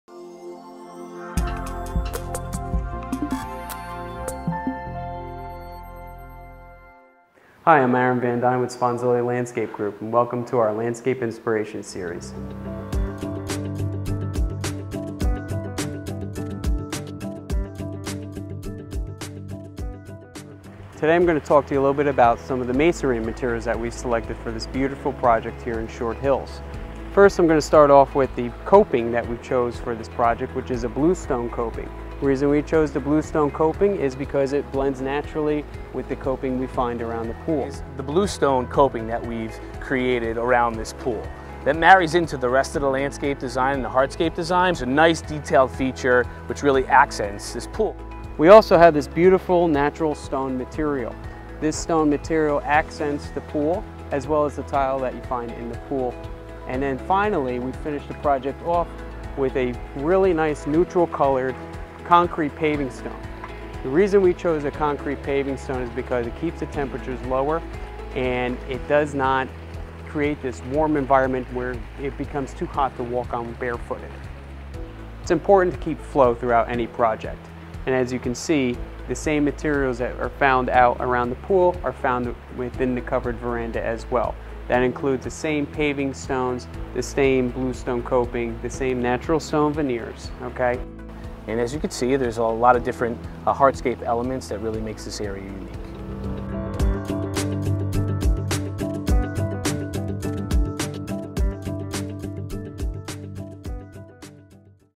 Landscape Stone & Masonry Materials - Sponzilli Landscape Inspiration Series
Complementary stone materials were utilized throughout this landscape design to bring together the different areas that were created. From the Blue Stone used on the retaining walls to the concrete pavers near the pool area, all materials were chosen to marry the hardscaping with the flow and feel of this resort inspired backyard design.